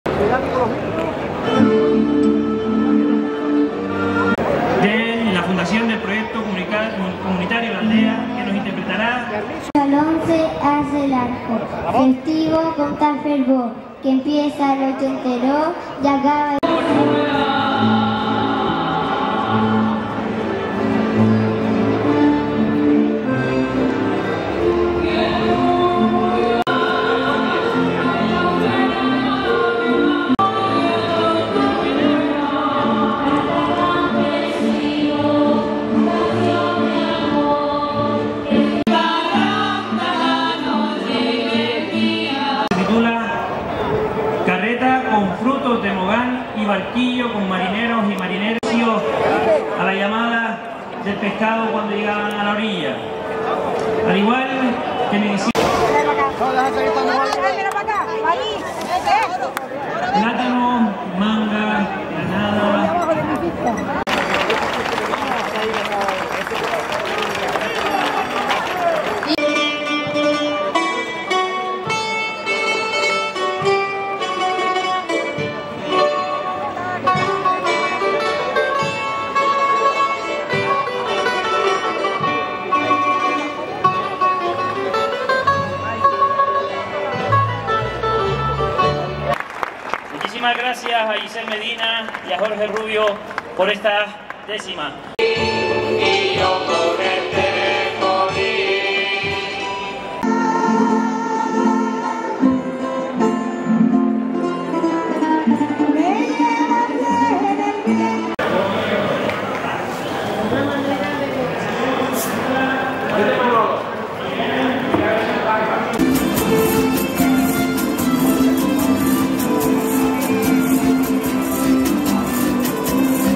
Toda Gran Canaria se da cita en la Romería-Ofrenda a la Virgen del Pino en Teror
Cerca de 20.000 personas han participado hoy en Teror en la Romería-Ofrenda a la Virgen del Pino. Las 22 carretas que siempre participan en esta cita, las 21 de cada uno de los municipios y la del Cabildo de Gran Canaria, partieron desde el Castañero Gordo hasta llegar a las puertas de la Basílica, donde se encontraba la Virgen, a la que ofrecieron lo mejor de sus campos, acompañadas todas las carretas por el folclore y los bailes tradicionales.